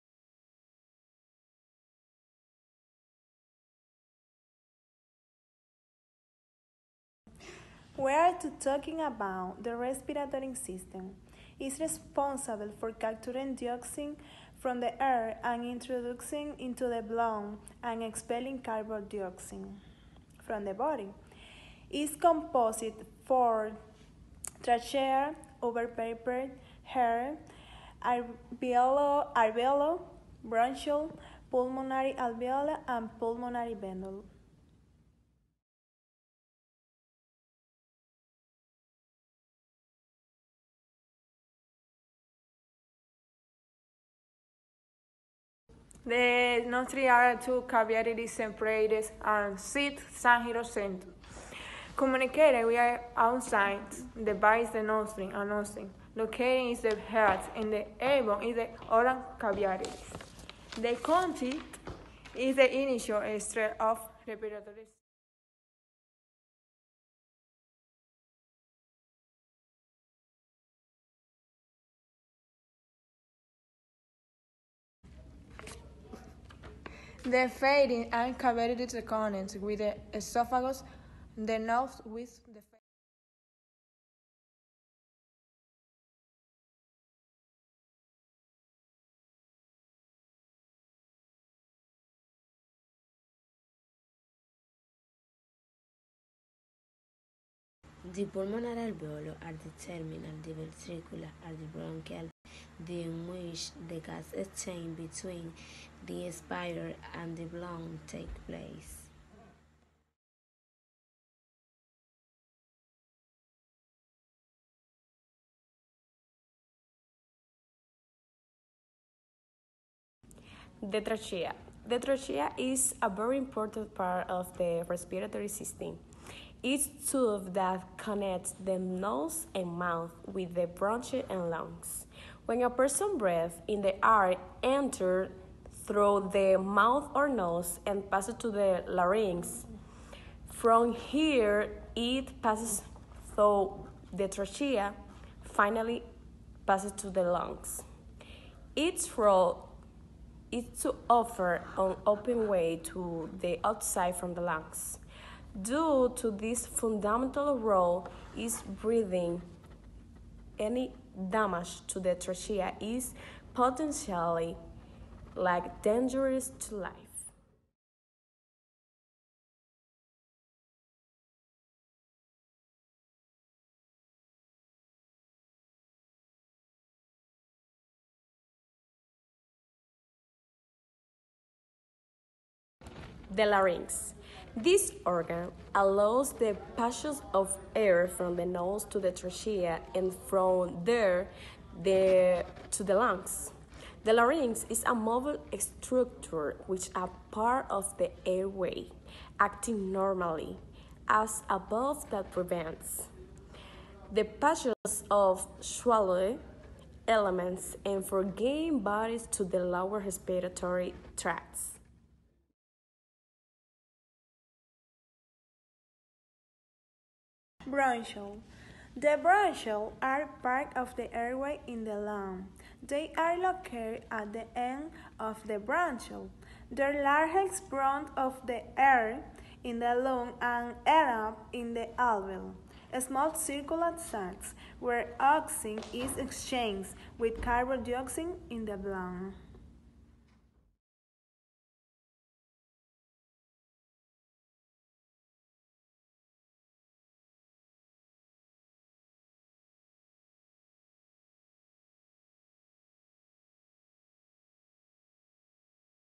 Presentación del sistema respiratorio en inglés, por estudiantes de medicina.
Video de Celia Castillo